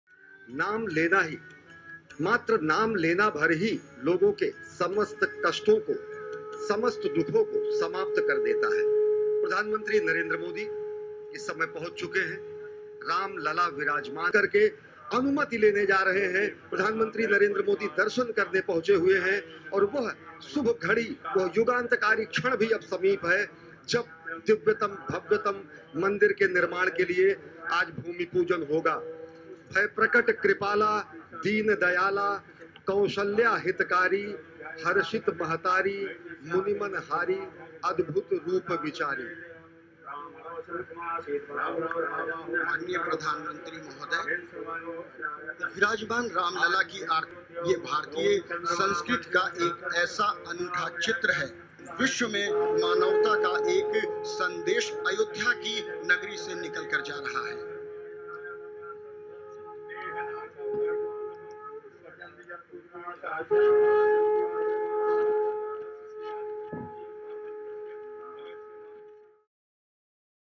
India: Masked Modi prays at controversial temple site | AFP
Indian Prime Minister Narendra Modi, wearing a white mask, offers prayers ahead of a ceremony to lay the foundation for a temple at a flashpoint holy site in the northern city of Ayodhya.